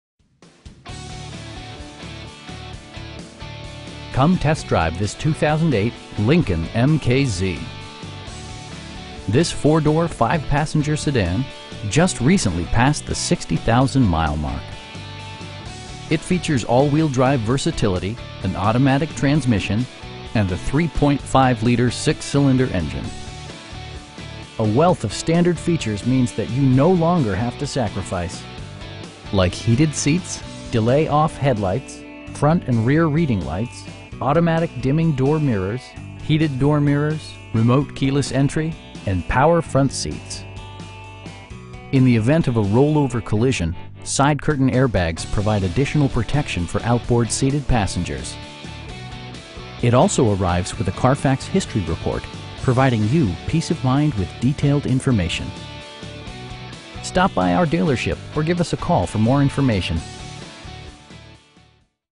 2008 LINCOLN MKZ in South Burlington, VT 05403
Willie Racine's Jeep

Come test drive this 2008 Lincoln MKZ. This 4 door, 5 passenger sedan just recently passed the 60,000 mile mark! It features all-wheel drive versatility, an automatic transmission, and a 3.5 liter 6 cylinder engine. A wealth of standard features means that you no longer have to sacrifice. Like heated seats, delay-off headlights, front and rear reading lights, automatic dimming door mirrors, heated door mirrors, remote keyless entry, an overhead console, and power front seats. In the event of a rollover collision, side curtain airbags provide additional protection for outboard seated passengers. It also arrives with a Carfax history report, providing you peace of mind with detailed information. We pride ourselves on providing excellent customer service. Stop by our dealership or give us a call for more information.